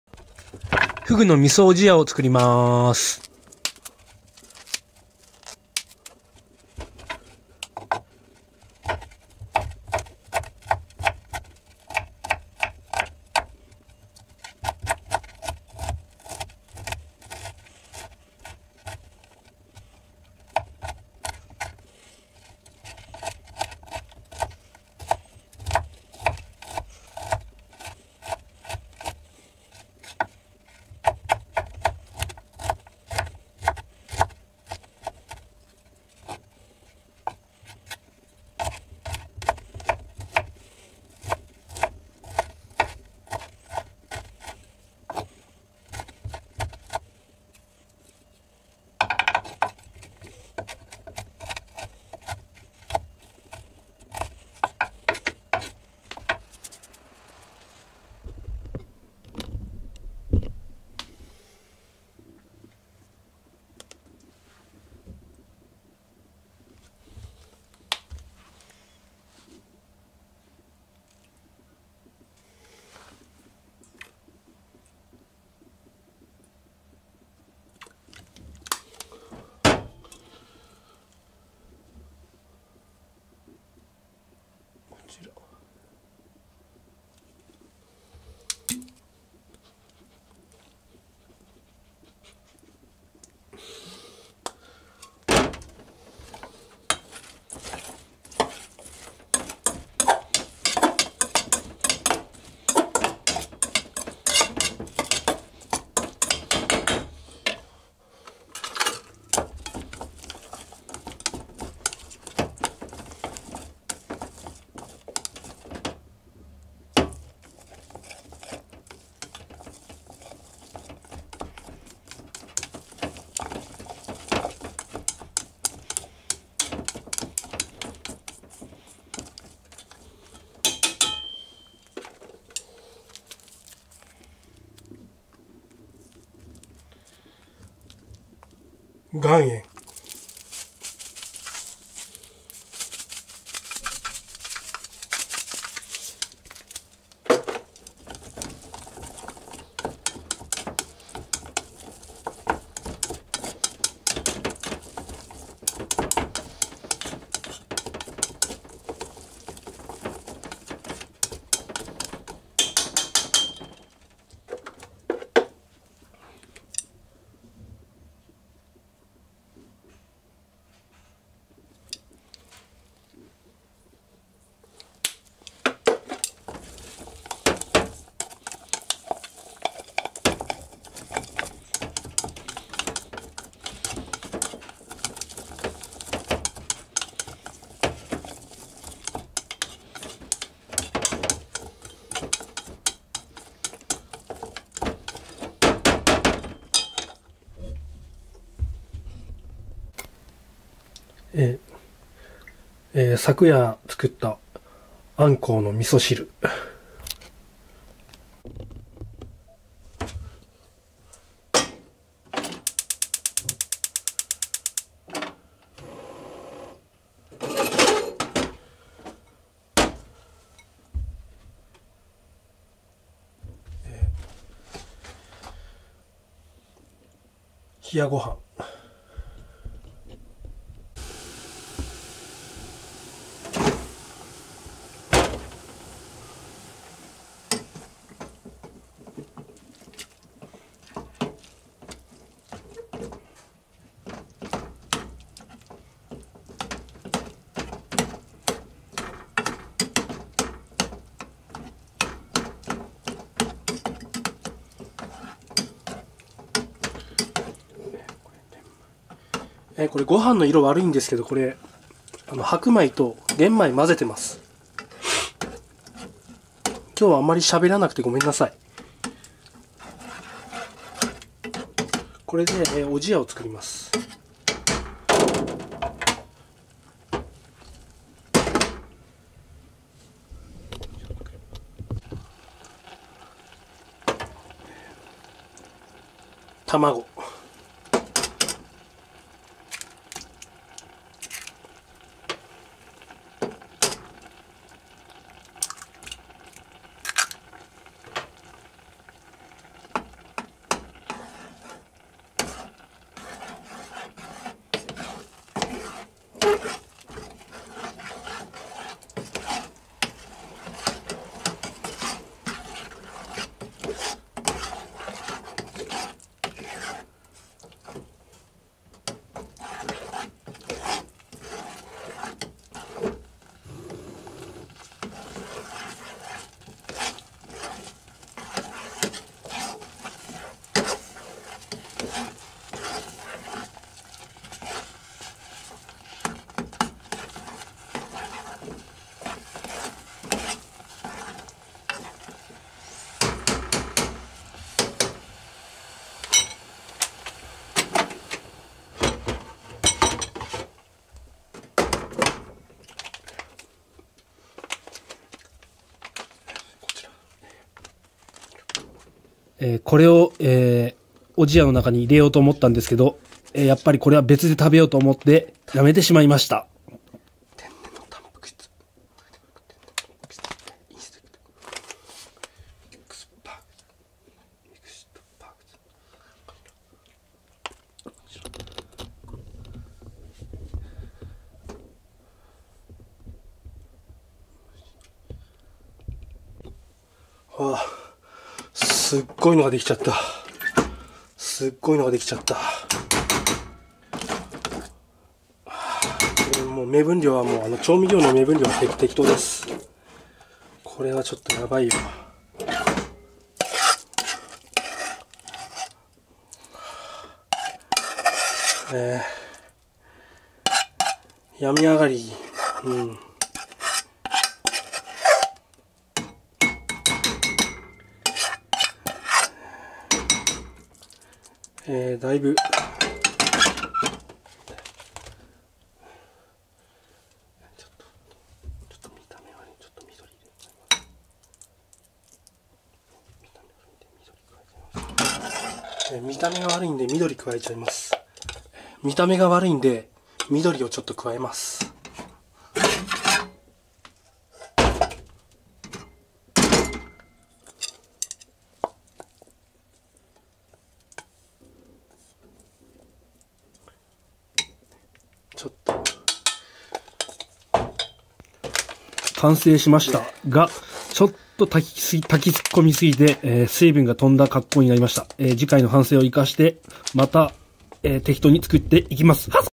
フグのバルクアップ味噌おじや
前日の晩御飯の残り物のフグでタンパク質カーボたっぷりの筋トレ飯を作る。